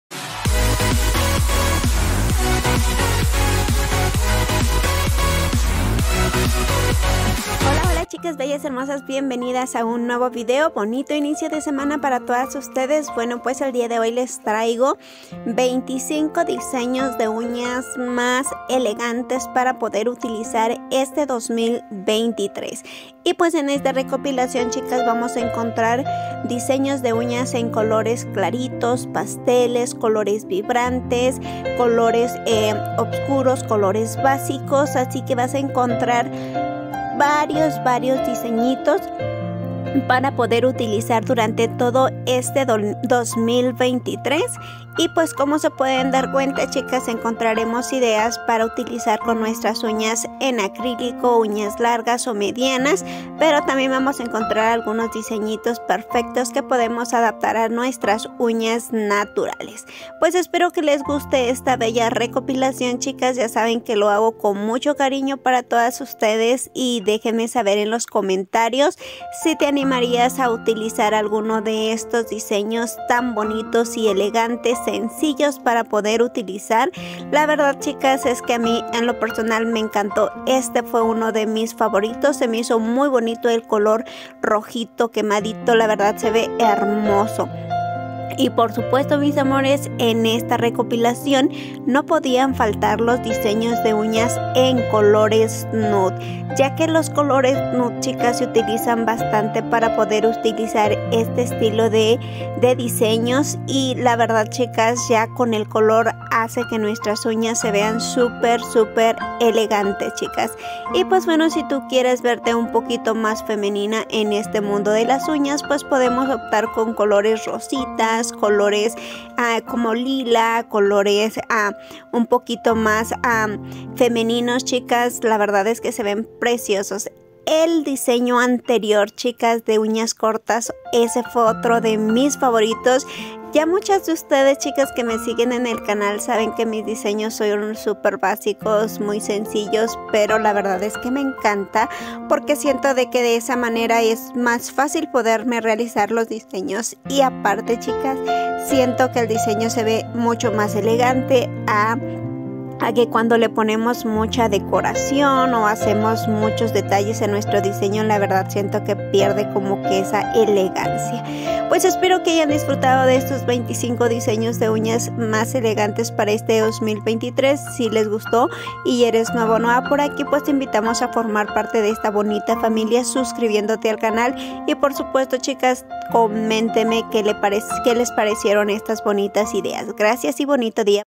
UÑAS ELEGANTE 2023/art nails Elegants
CHICAS HOY LES TRAIGO ÉSTOS 25 DISEÑOS DE UÑAS ELEGANTES PARA EL 2023 MUCHAS GRACIAS POR SU APOYO 😘 SALUDOS PARA TODOS Y NO SE PIERDAN EL SIGUIENTE VIDEO 💅